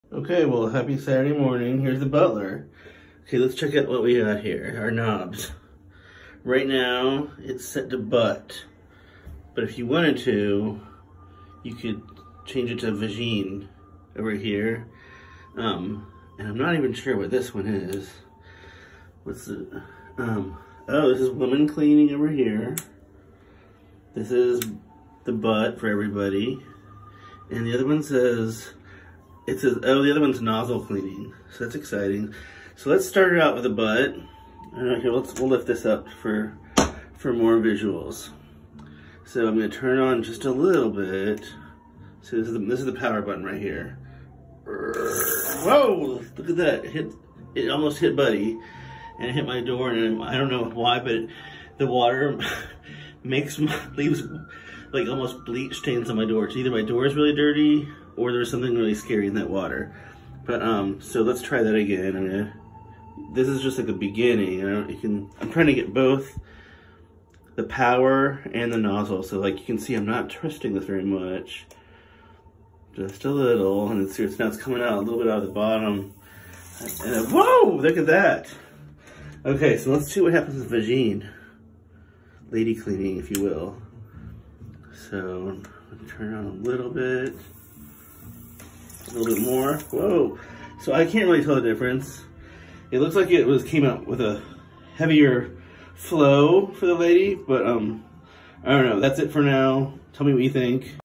The Buttler by Clear Rear
My demo of the Buttler by Clear Rear, toilet seat bidet.